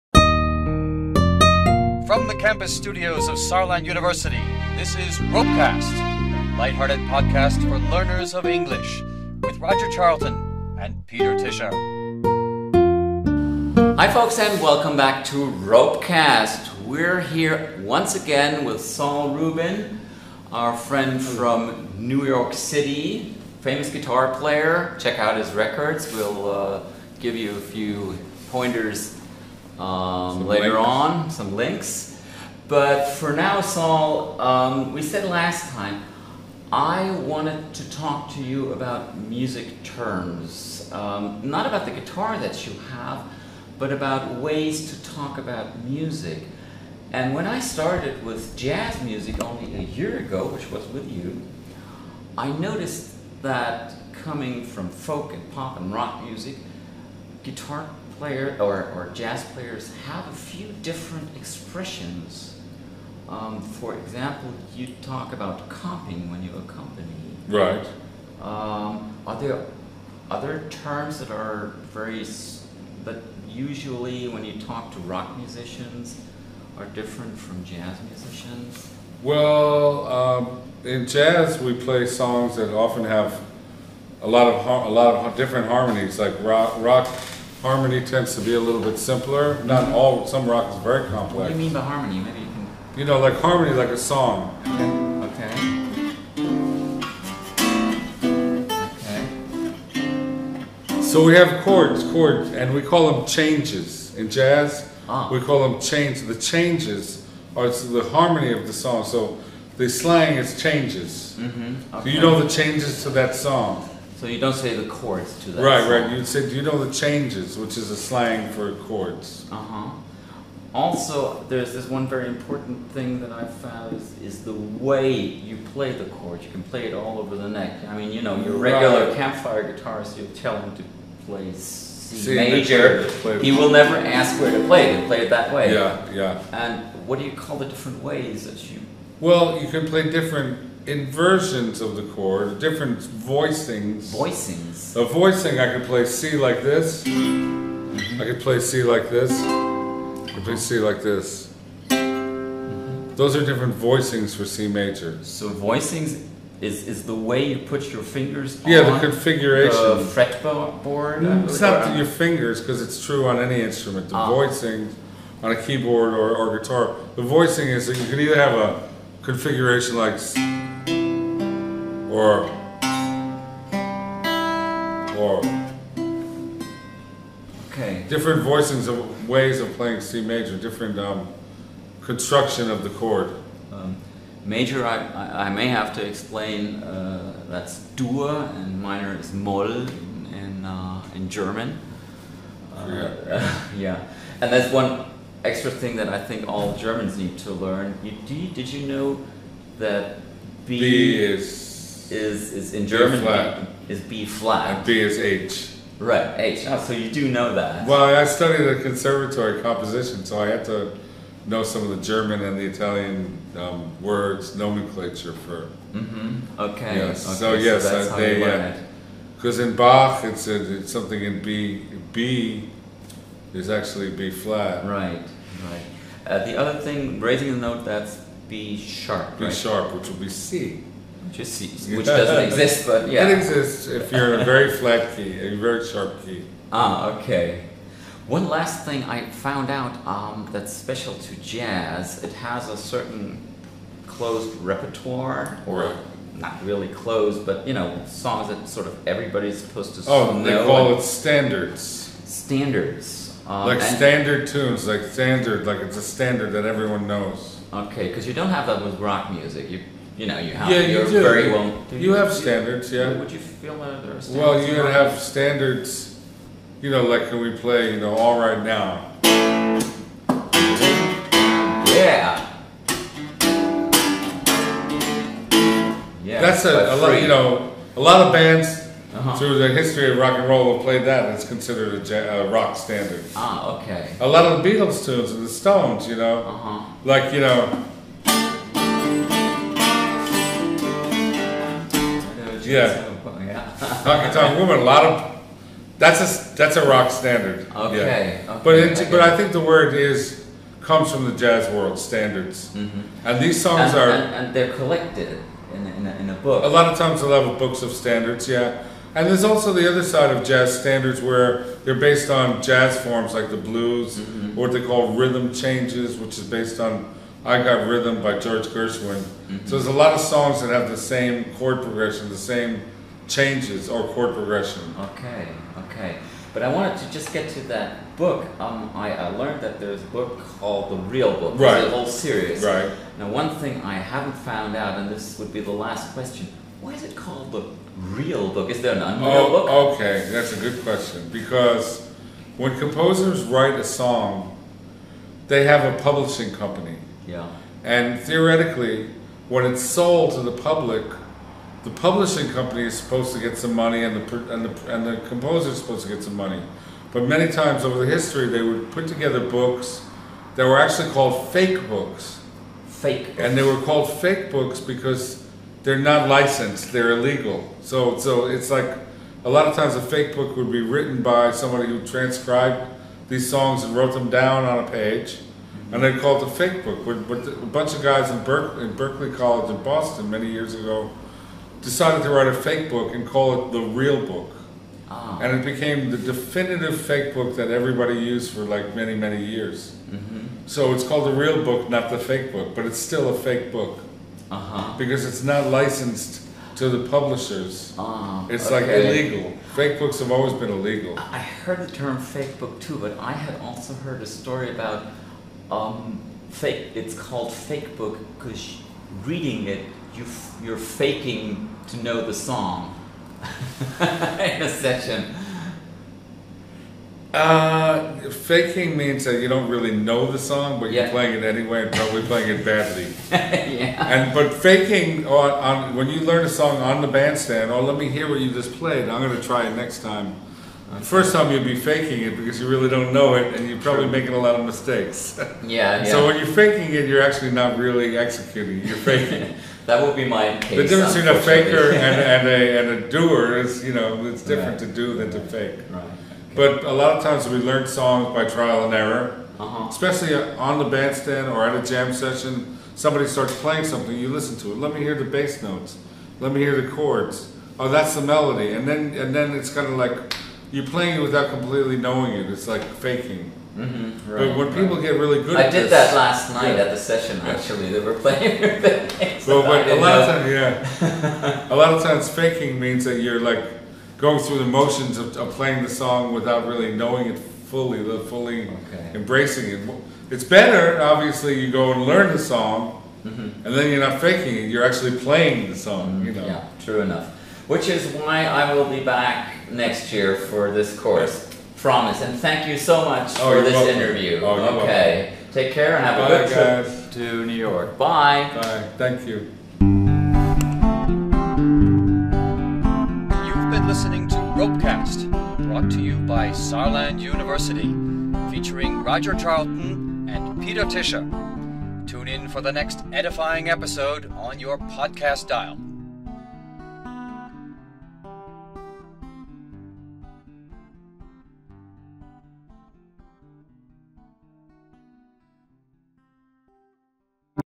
A Jazzy New Year! (RoPeCast #118)
Peter is back in jazzy 2018 for another episode with Saul Rubin, his guitar teacher from New York.

Today Peter asks about the jargon of Jazz music and how that differs from other styles' vocabulary. Talking about music is not an easy task afterall.

Finally, Saul can solve the mystery of the Real Book's name for Peter. Just listen in to learn how that name came about and why you could be faking a song if you play it from the Real Book.